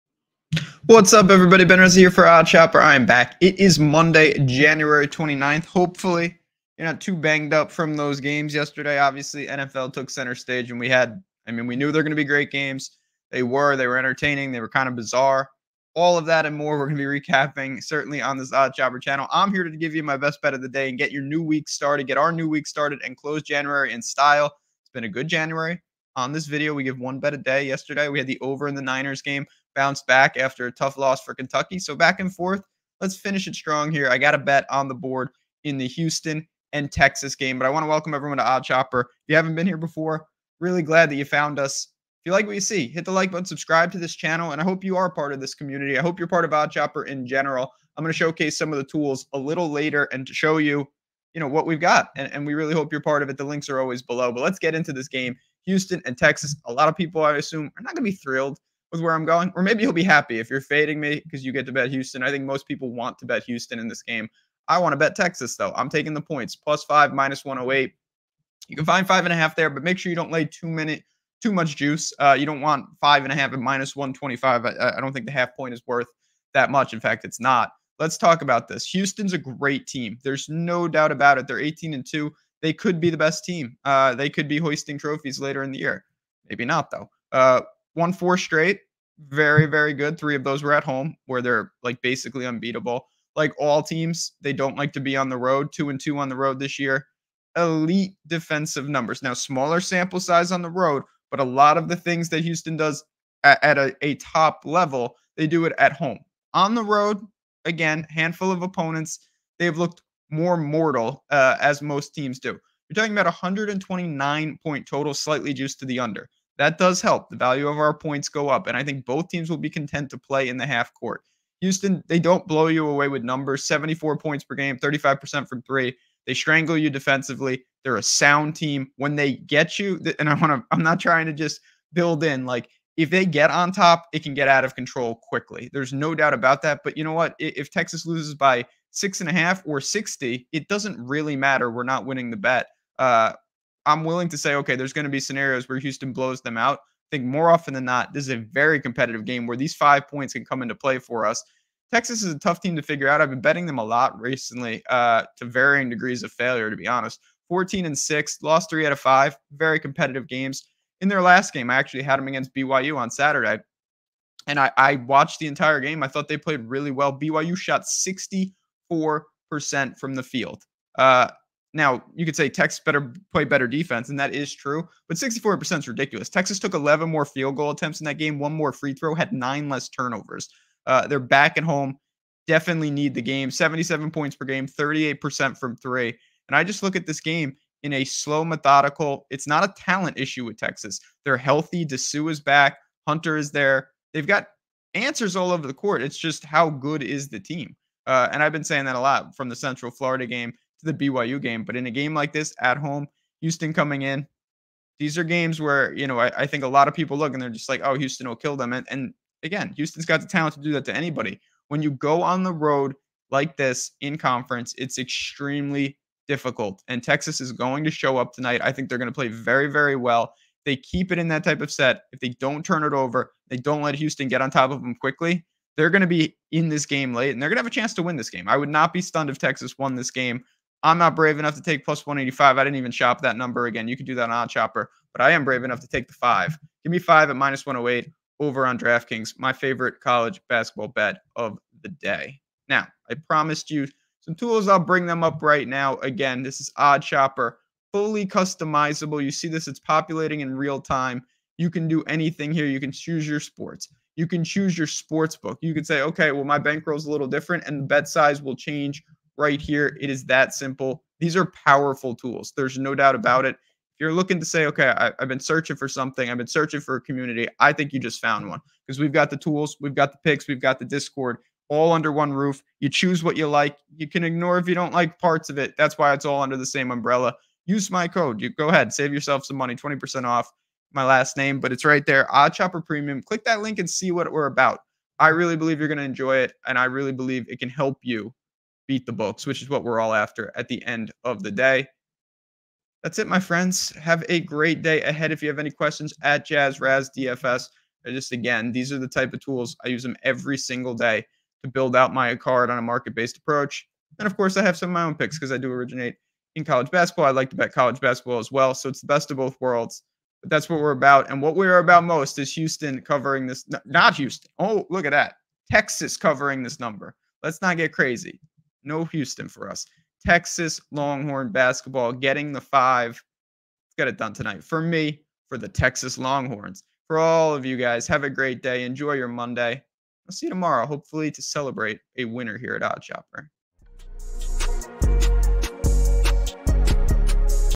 Best College Basketball Pick Today (1/29/24) Houston at Texas | Ben's Sharp Bet
💰Best Bets for Today💰

00:00 Best Bets for Today
1:00 Houston at Texas
4:00 Bonus Bets